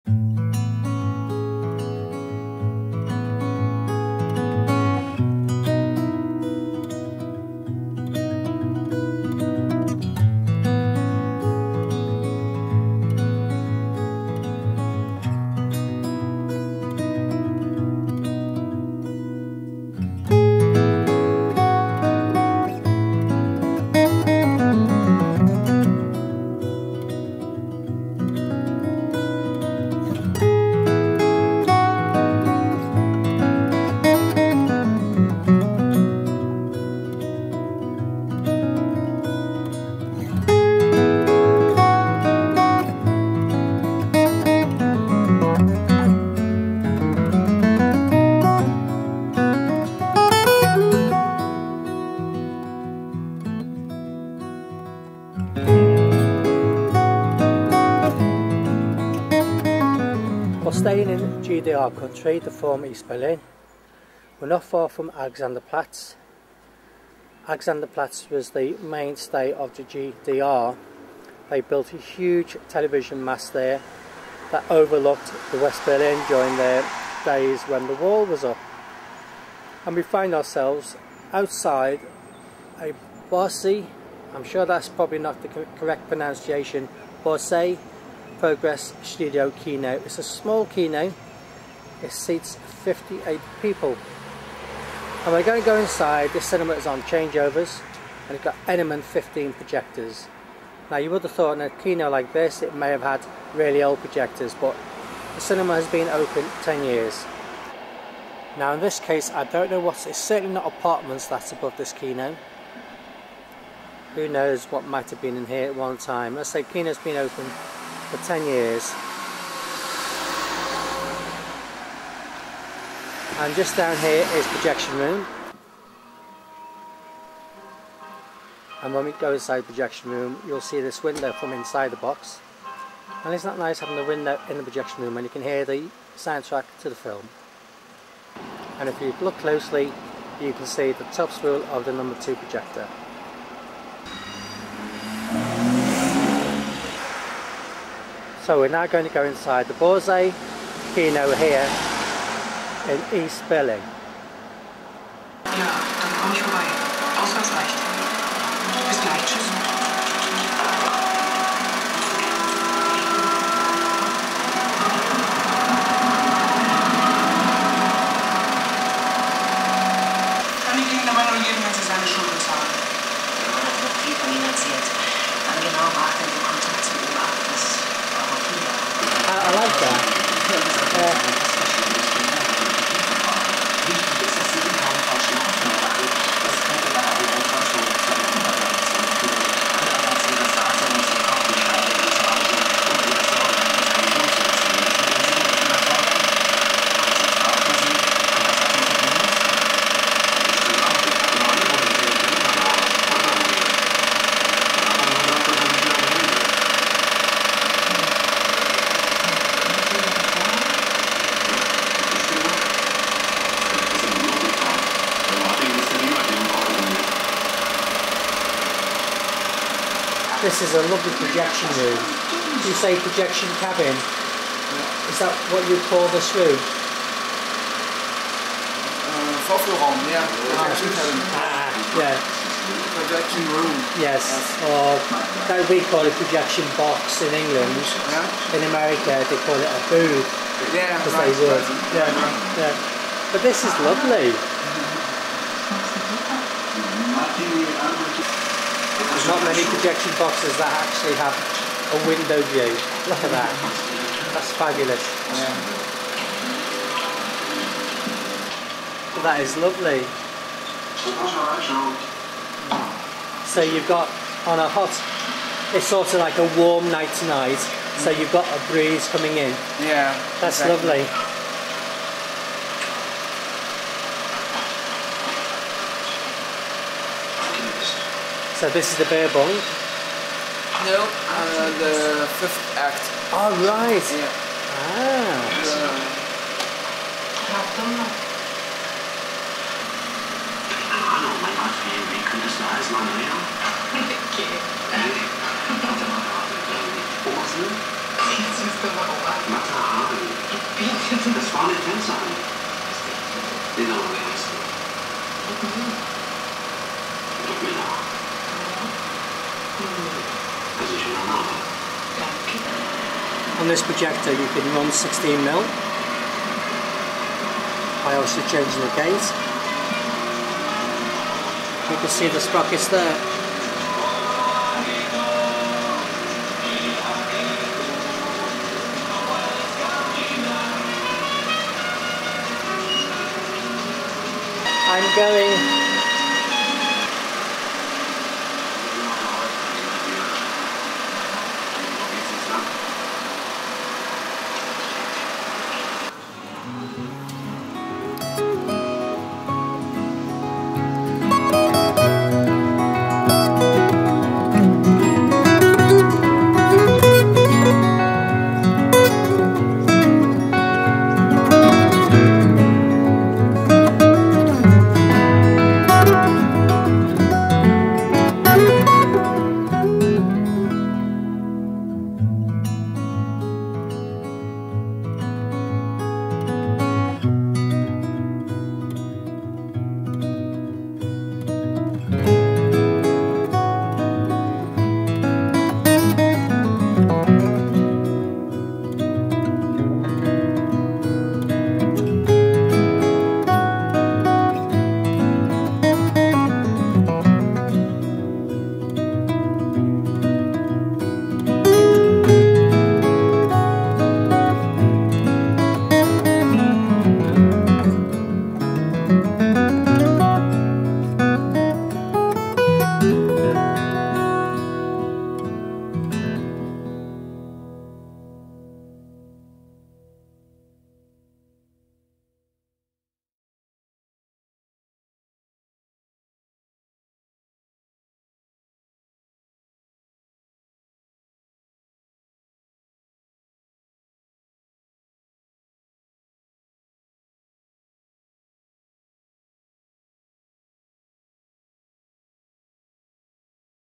BORSE KINO FORMER EAST BERLIN
This is a small but very interesting Kino. It's a single screen seating about 58 with a nice auditorium. It's in GDR country . It normally used to show German and Russian films. Sadly it is no longer with us. This makes a perfect entry into "Memories of Cinema"